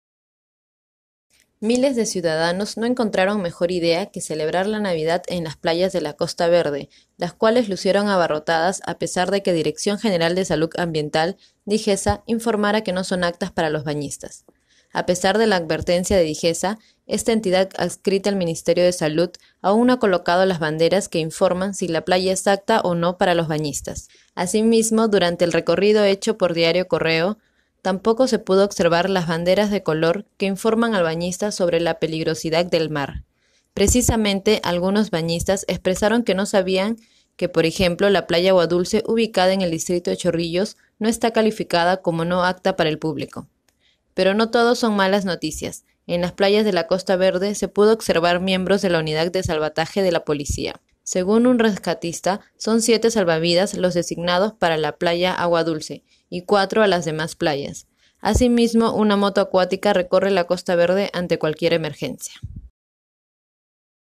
BAÑISTAS CELEBRAN NAVIDAD EN PLAYAS DE LA COSTA VERDE
UN DÍA DESPUÉS DE LA NOCHE BUENA, MILES DE LIMEÑOS APROVECHARON EL FERIADO PARA ACUDIR A LAS PLAYAS DE LA COSTA VERDE, PESE A QUE LA DIRECCIÓN GENERAL DE SALUD INFORMARA QUE AÚN NO ESTÁN APTAS PARA EL PÚBLICO.